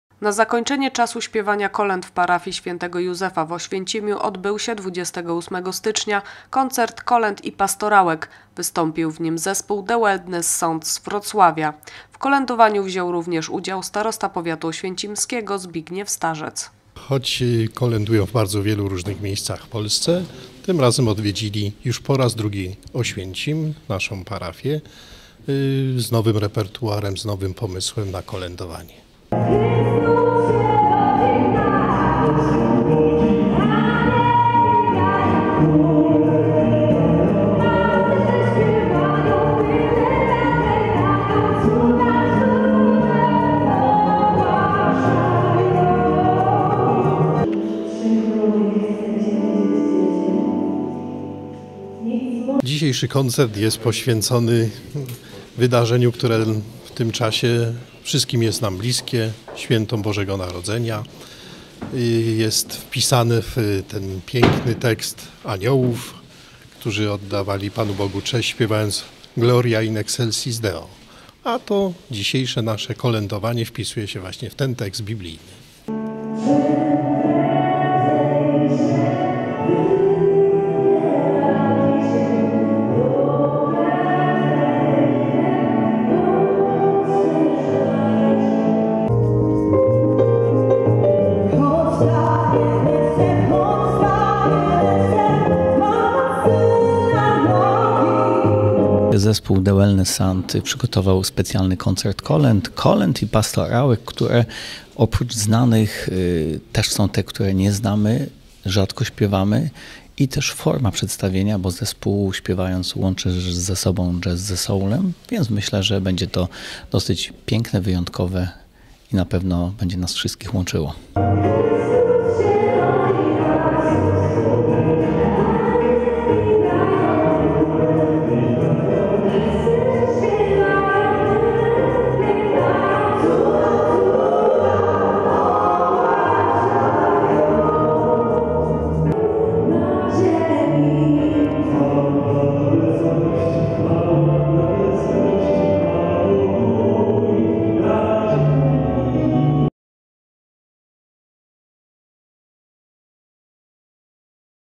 Koncert w parafii św. Józefa w Oświęcimiu
Na zakończenie czasu śpiewania kolęd w parafii św. Józefa w Oświęcimiu odbył się 28 stycznia koncert kolęd i pastorałek, wystąpił w nim zespół: "The Wellness Sound" z Wrocławia. W kolędowaniu wziął również udział starosta powiatu oświęcimskiego Zbigniew Starzec.